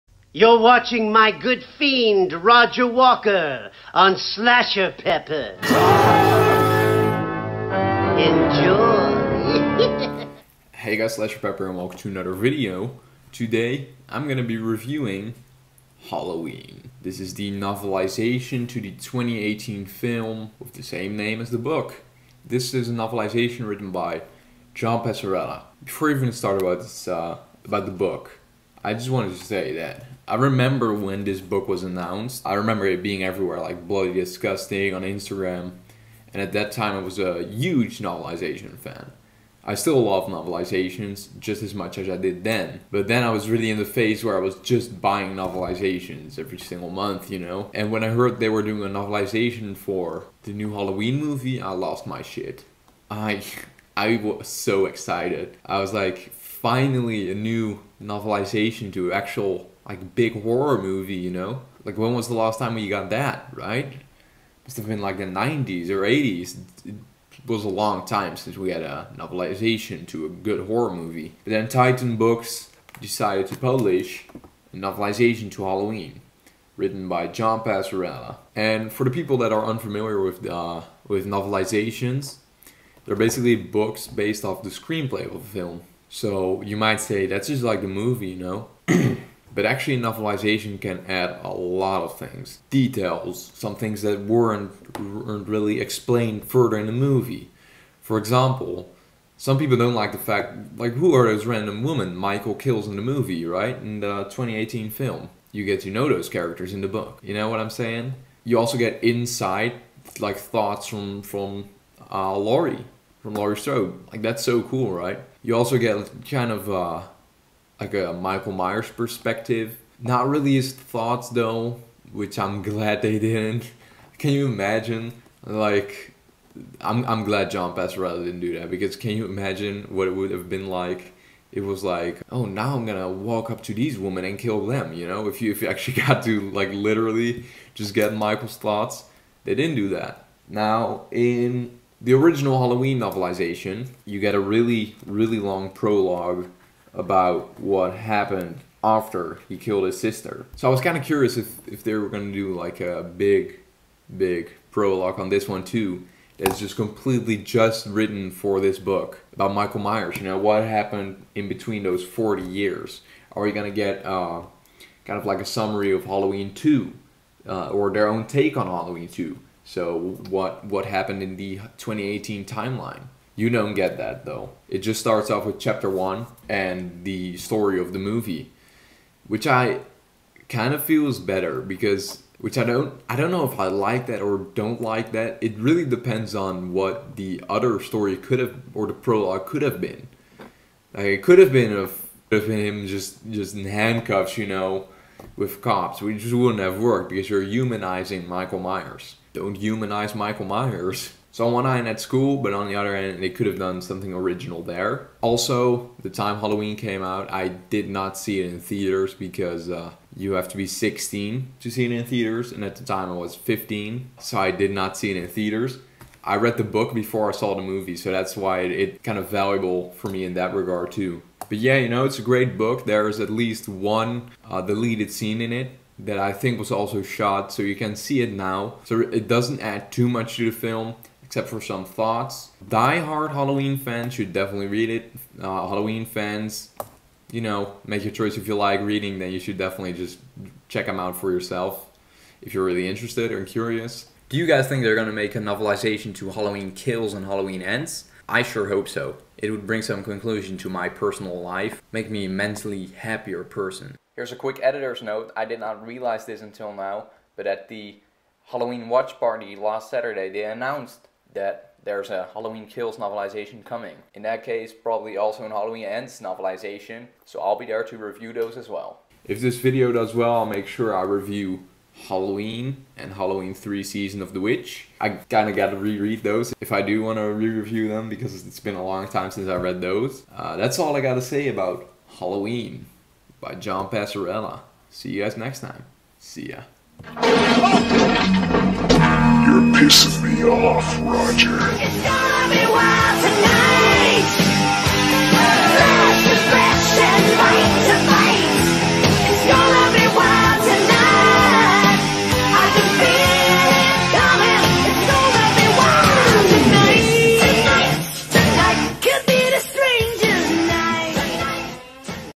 Halloween (2018) John Passarella Novelization Review
In this video I'll be reviewing Halloween the book by John Passarella. This is the book based of the screenplay which marked the return of Michael Myers. Enjoy...